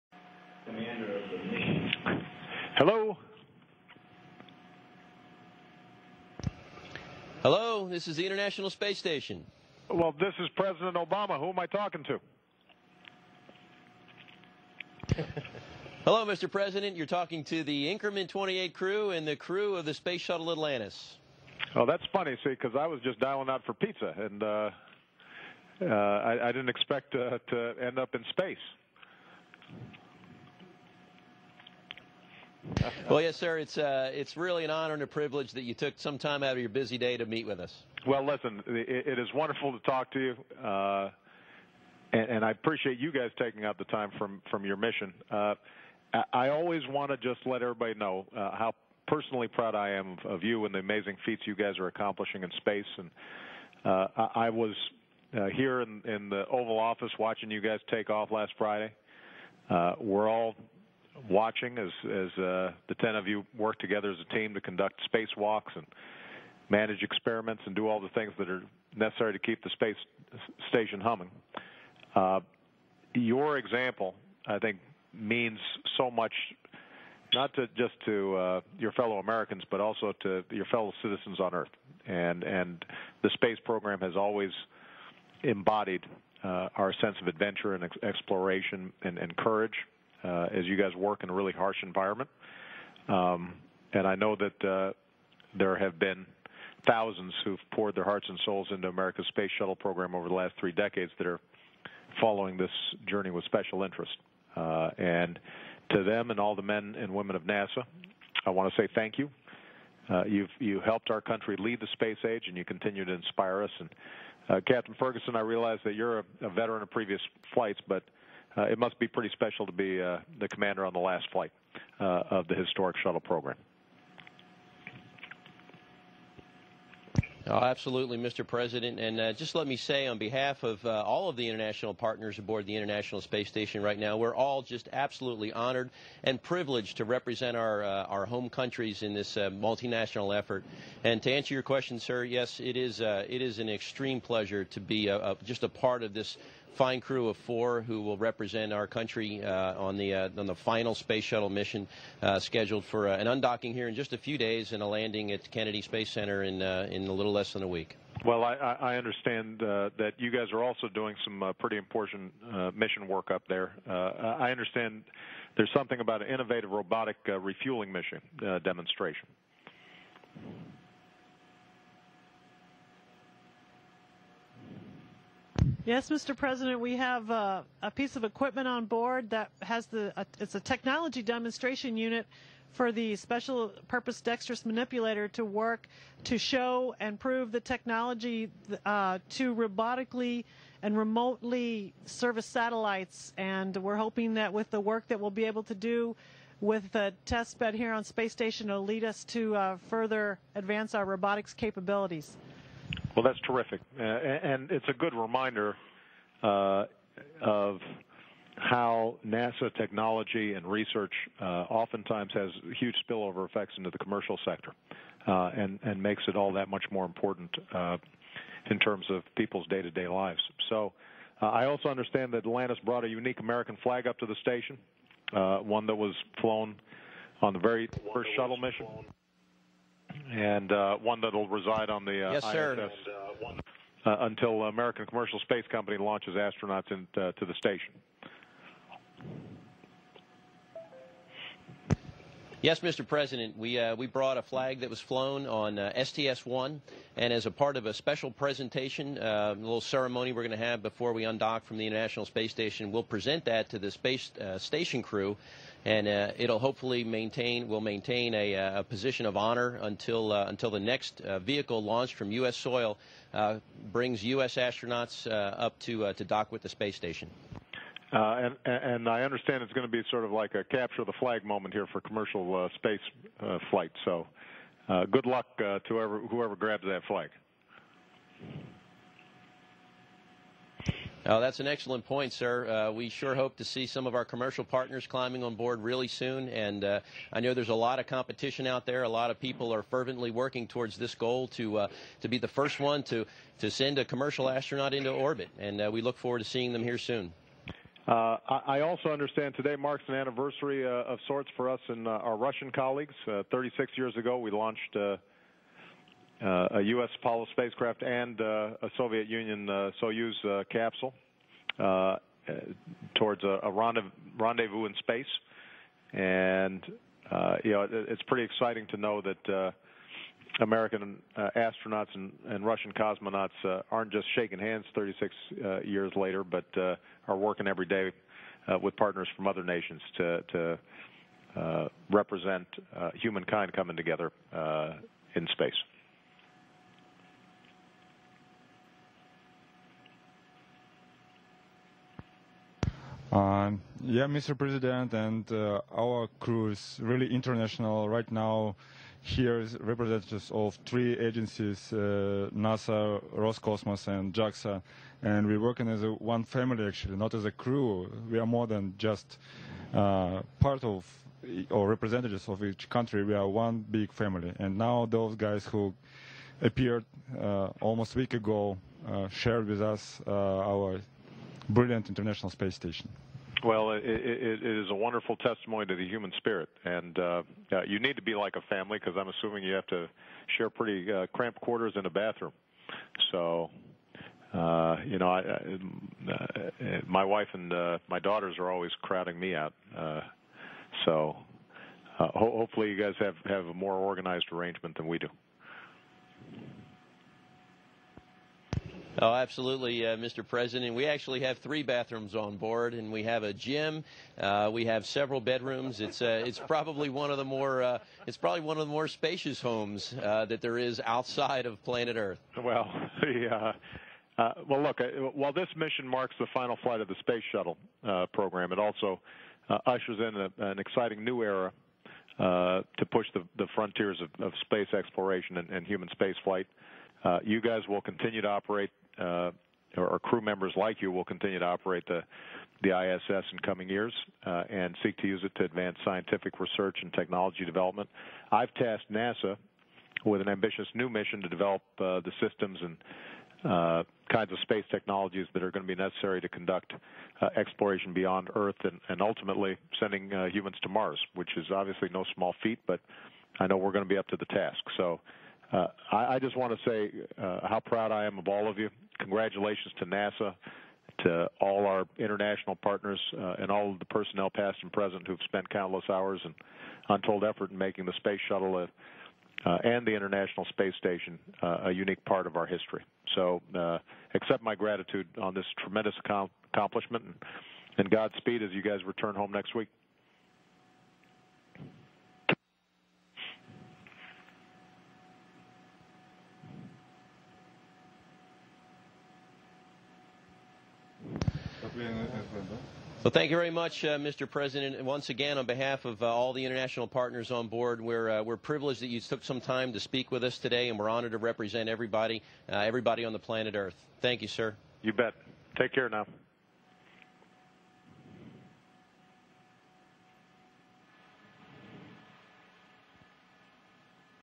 Obama Talks Future in Call to Space Station
President Barack Obama spoke with astronauts about the legacy of the Space Shuttle and his commitment to Humanity's future in space. This call comes against a background of turmoil between NASA and the US Congress.